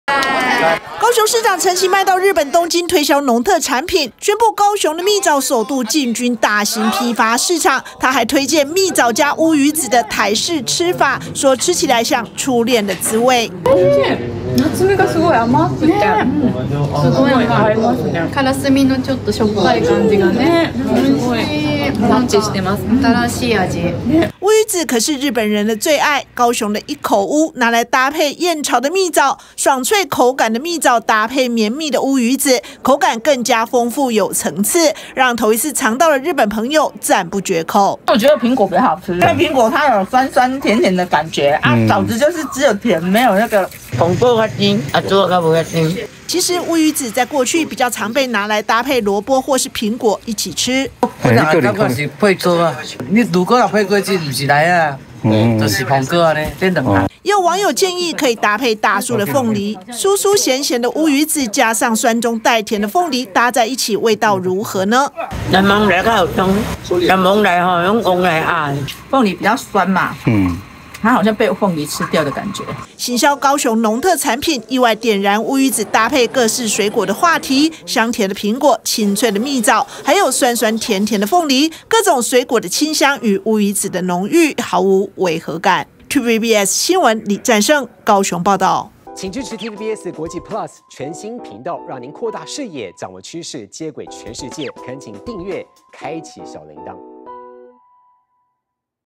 陳其邁推「烏魚子搭蜜棗」 日本人讚不絕口｜TVBS新聞@TVBSNEWS01
高雄市長陳其邁前往日本東京宣傳高雄農特產，特地向日本人使出蜜棗加烏魚子的台式吃法，陳其邁說，吃起來像「初戀的滋味」，這爽脆口感的蜜棗，搭配綿密的烏魚子，口感豐富有層次，讓頭一次吃到的日本朋友驚豔不已！結果，意外掀起烏魚子到底搭配什麼水果最對位的話題。